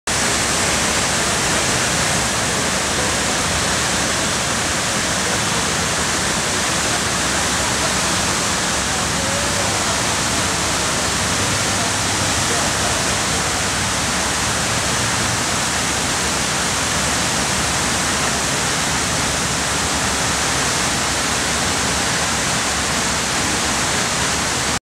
World Trade Center 911 Memorial
A look at the new tower memorial.  The waterfall of the south tower at the WTC,